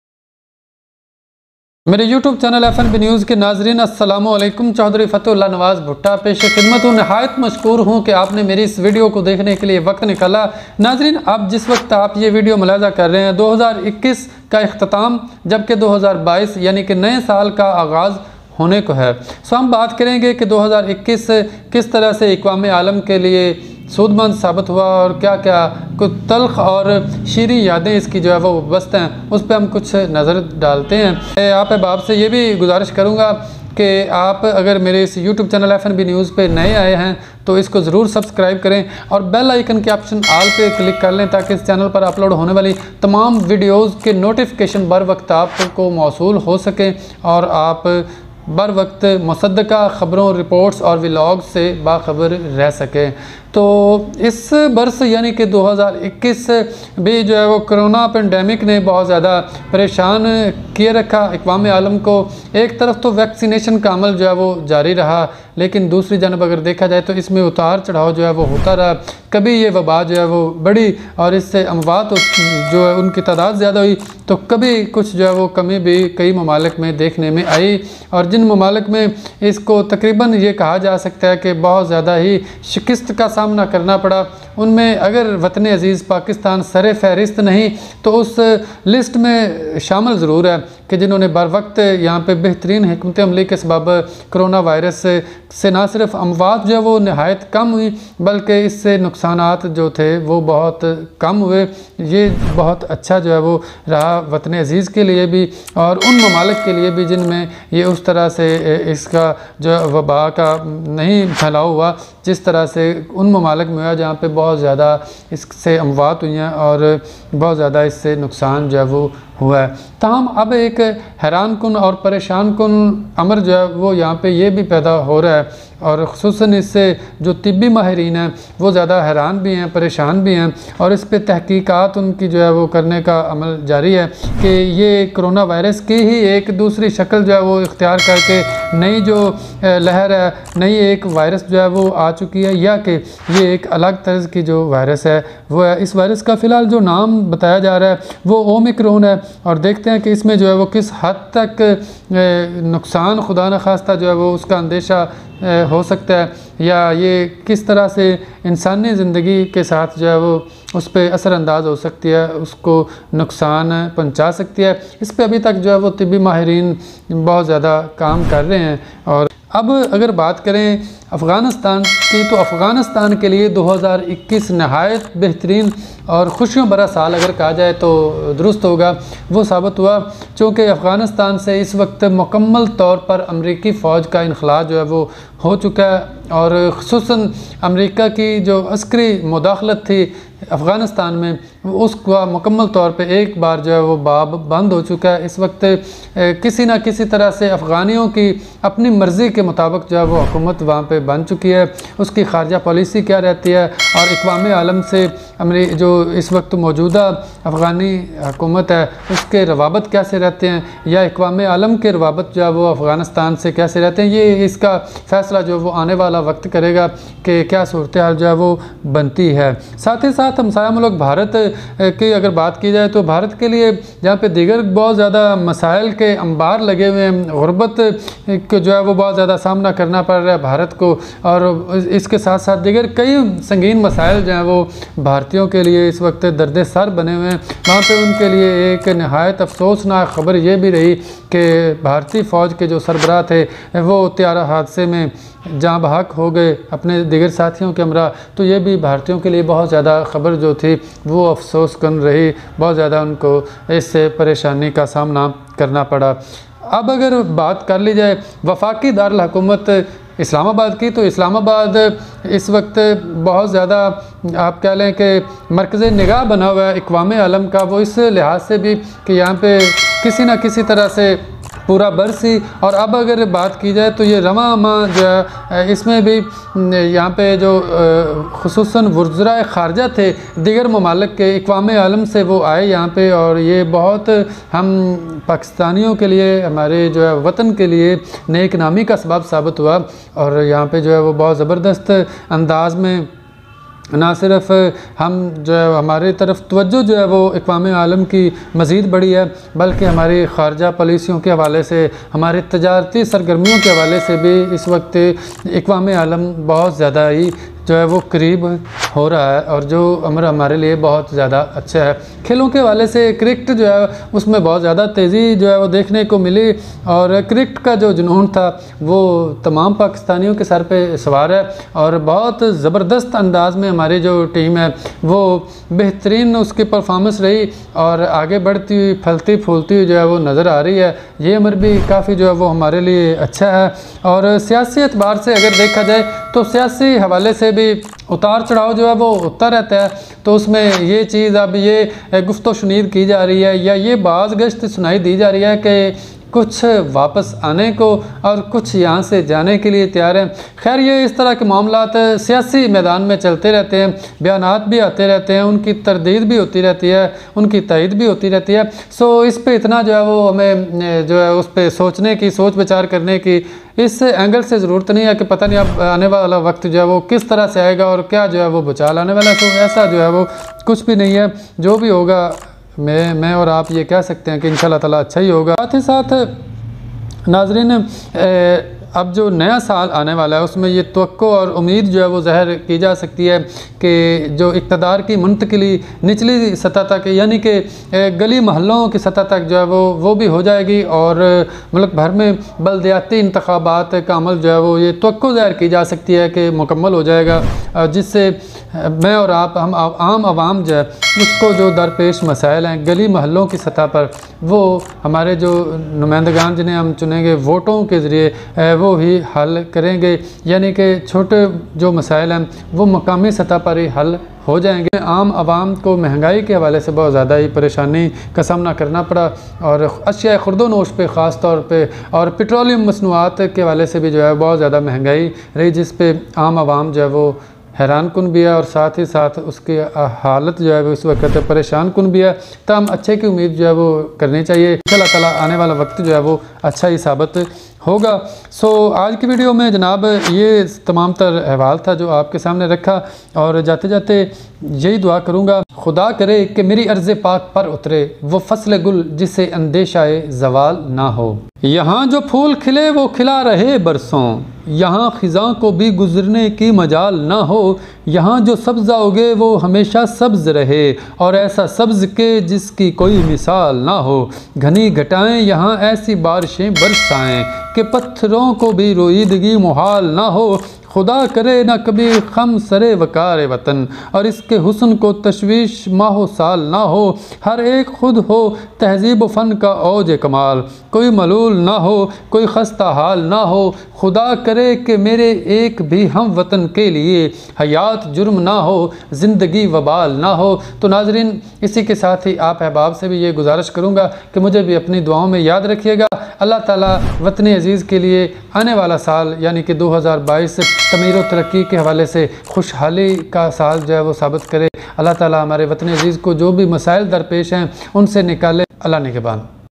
2021میں دنیا کو کن چیلنجز کا سامنا کرنا پڑا ؟ | FnbNews | HappyNewYear | چوھدری فتح اللہ نواز
In this video, find out the details of what the beloved homeland and the United Nations lost and found in 2021